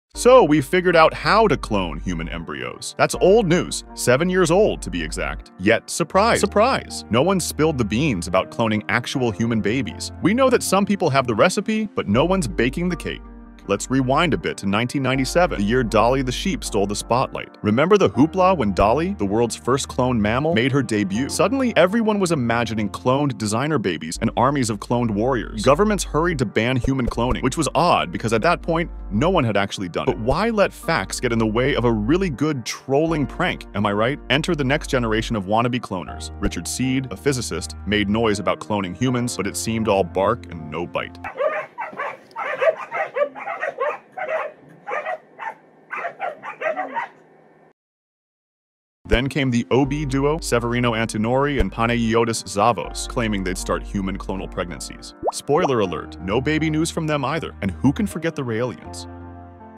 Where are we now in the World of Human Cloning?
Seven years ago, scientists successfully cloned human embryos. Let's look where we're at right now.